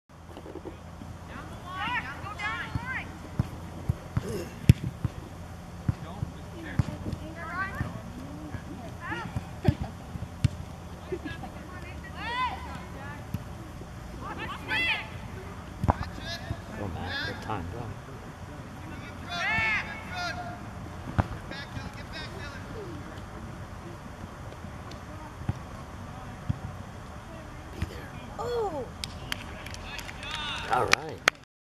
HMS Soccer - Willington vs Columbia (Ryan)
9/29/14   Matt's assist to Ryan heading the ball into the goal
WIN 5-3